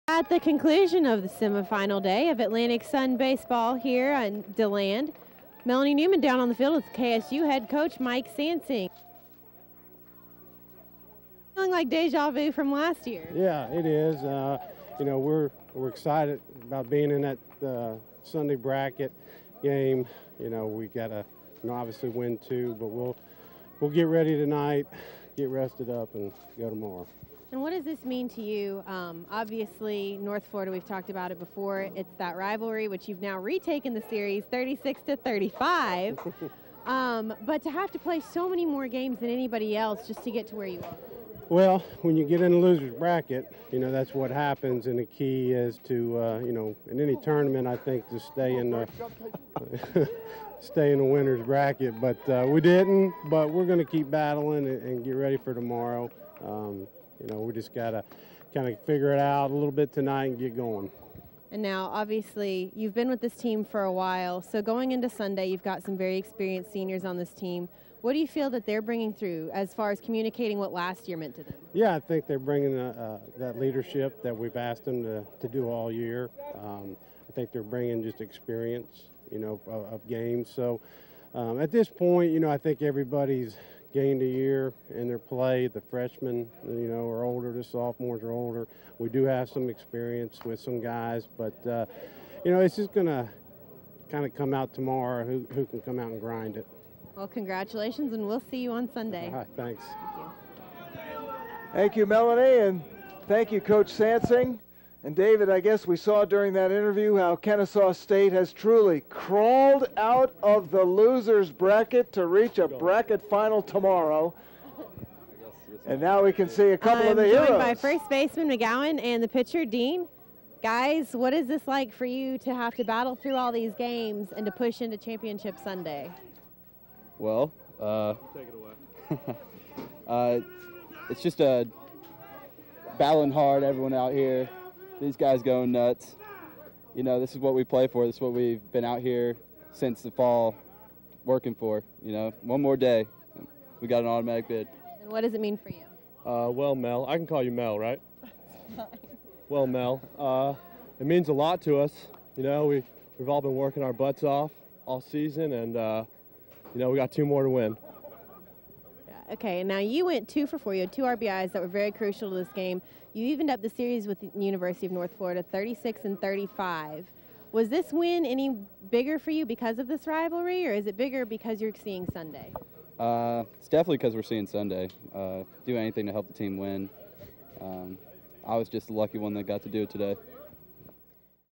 Sansing, McGowan, Dean; Kennesaw State: A-Sun Championship
Kennesaw State's head coach Mike Sansing, along with Chris McGowan and Travis Dean, talked about the Owls' victory over North Florida that advanced the team to championship Sunday of the 2013 Atlantic Sun Baseball Championship.